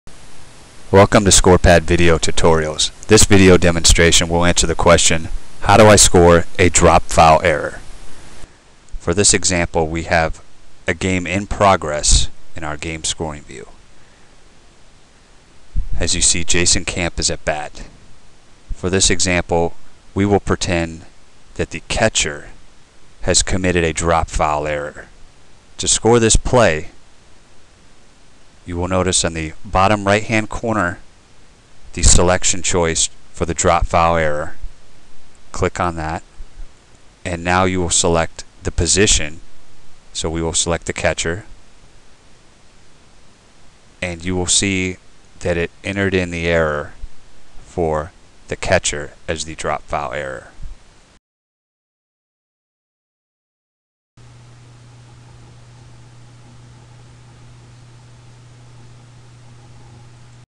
Scoring a Dropped Foul Error
How to score a dropped foul error using ScorePAD software.